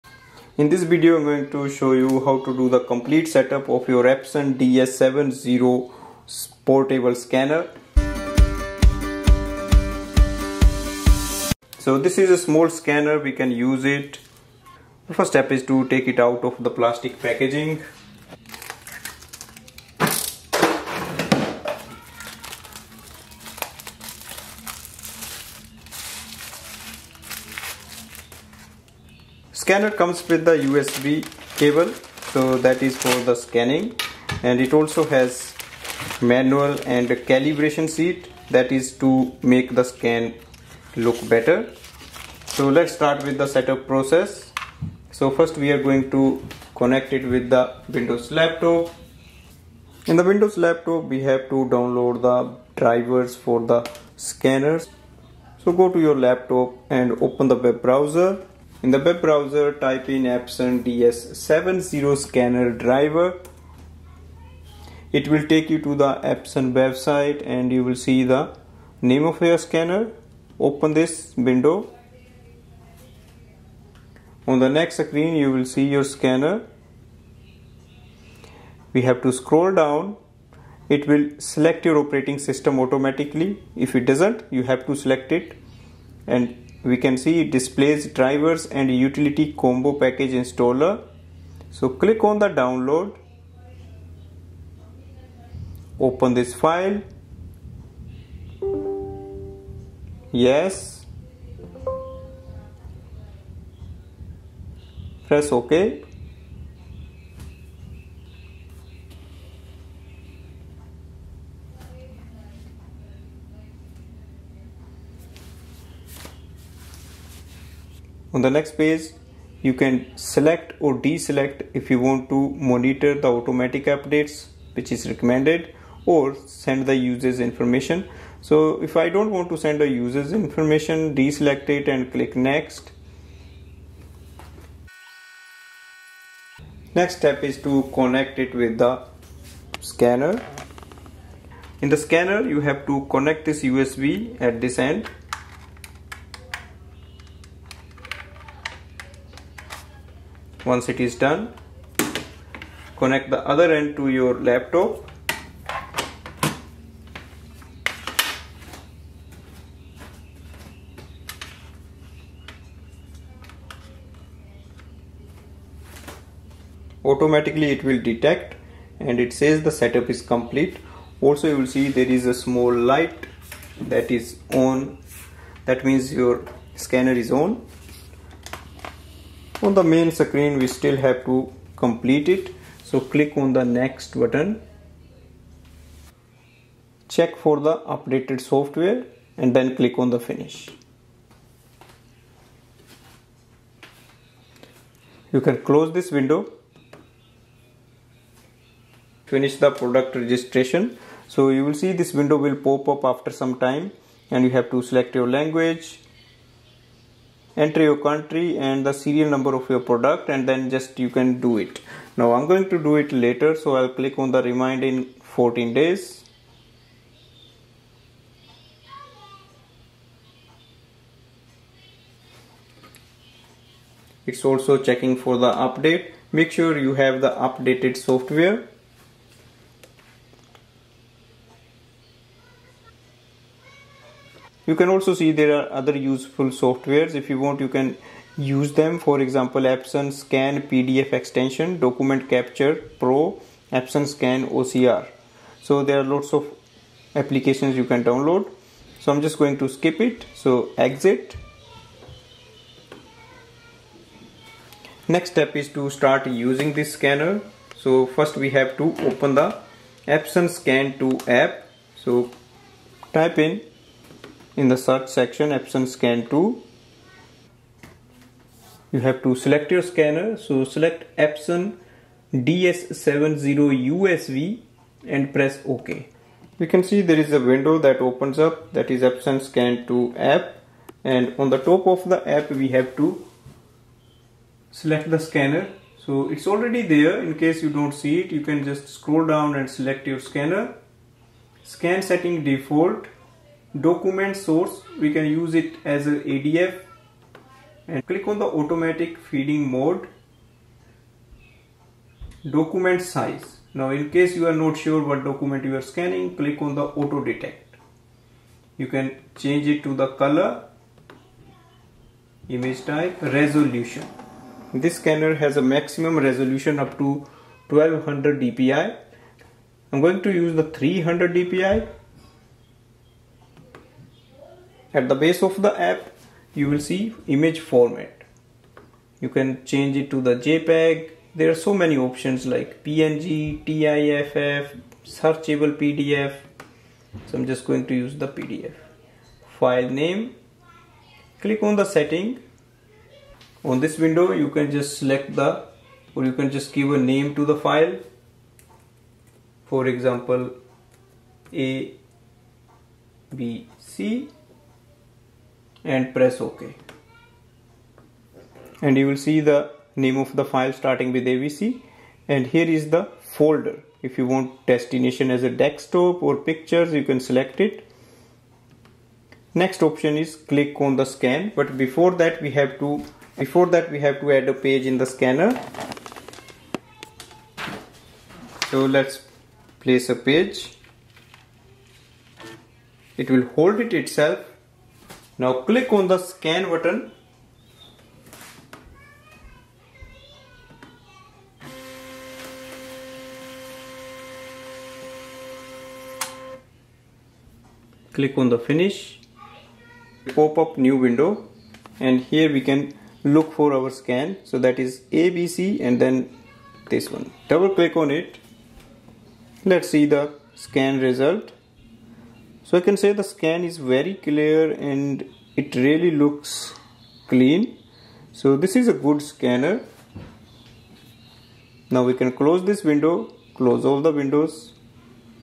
Epson DS70 Scanner Setup With Windows Laptop or PC, Install Drivers, Scanning Test !
This video shows the complete usb cable setup of your Epson DS70 Scanner.
This video shows usb cable setup of the scanner and then adding drivers and softwares in the Windows laptop or PC and then using the scanner for  scanning documents or photos.